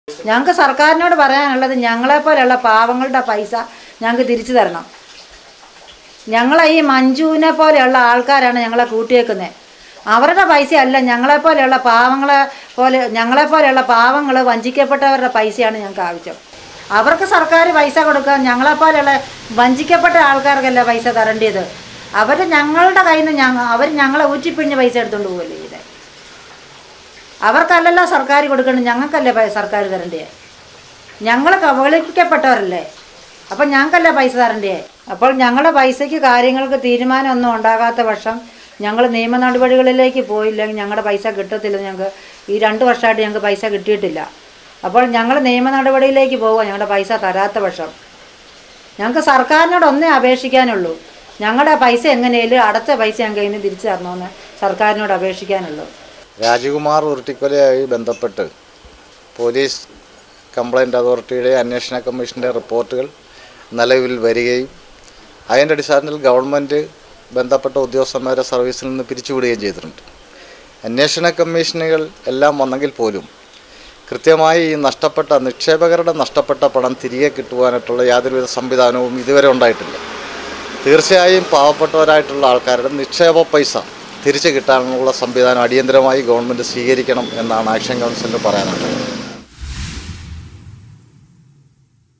ഹരിത ഫിനാന്‍സ് തട്ടിപ്പ് കേസ്; നിയമ നടപടിക്കൊരുങ്ങി തട്ടിപ്പിനിരയായവർ
സ്വാശ്രയ സംഘങ്ങൾക്കും വ്യക്തികൾക്കും ലക്ഷങ്ങൾ വായ്‌പ നൽകാമെന്ന് പറഞ്ഞാണ് തൂക്കുപാലം കേന്ദ്രമാക്കി തട്ടിപ്പ് നടത്തിയത്.